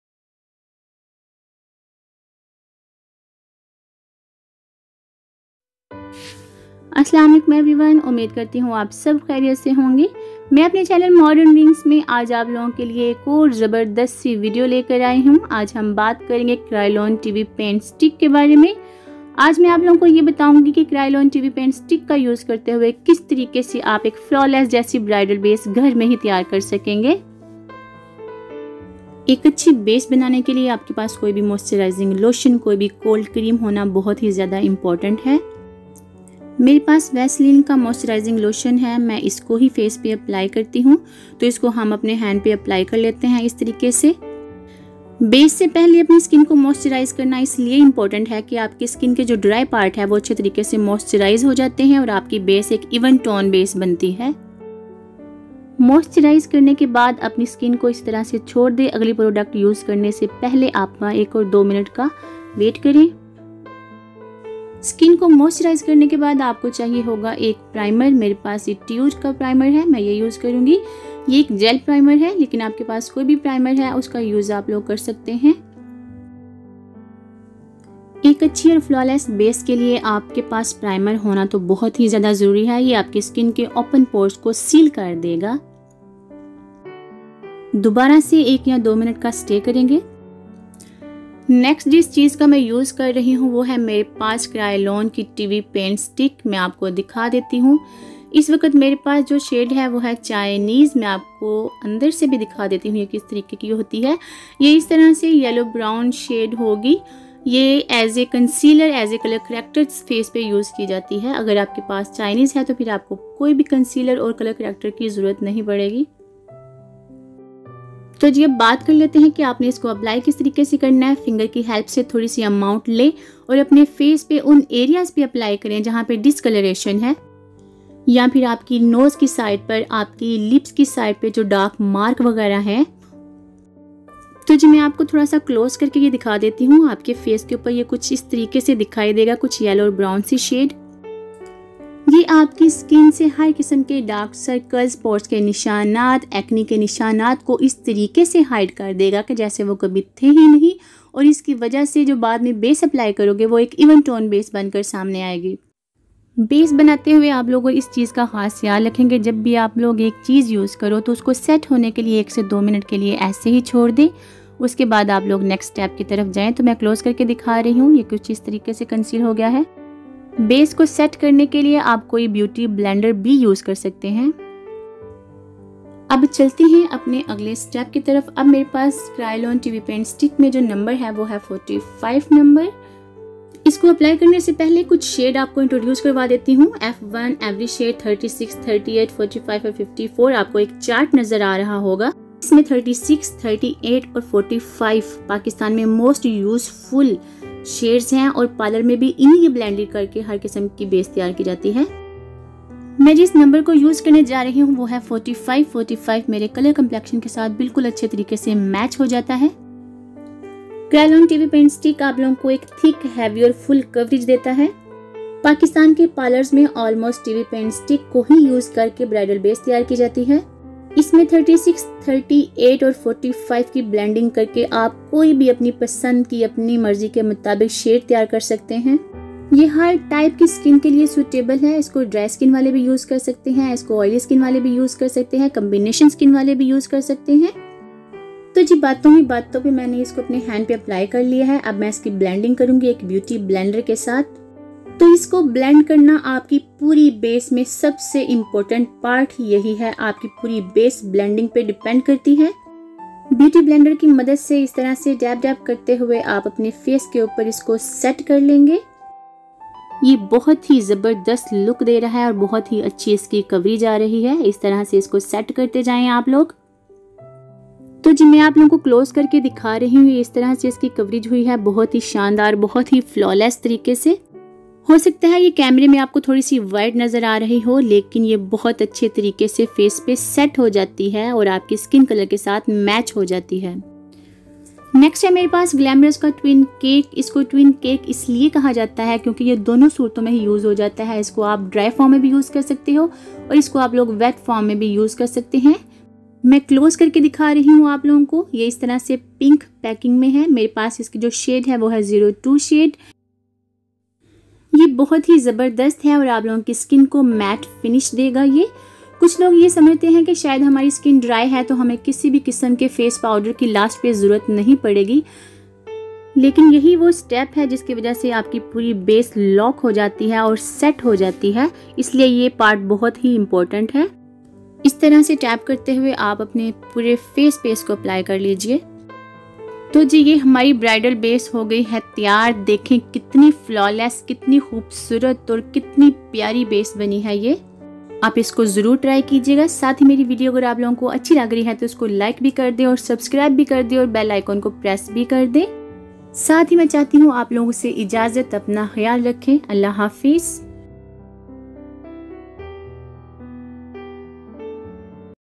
How to make bridal base | Bridal base at home | Cryalon TV paint stick | How To Apply Bridal Base
HI GUYSSS! Today's video is all about using Kryolan TV Paint Sticks and how to apply bridal base using these sticks. Hope you find this video helpful

In this video I have told about cryalon tv paint stick you can learn how to apply bridal base at home . Stay away Happy and healthy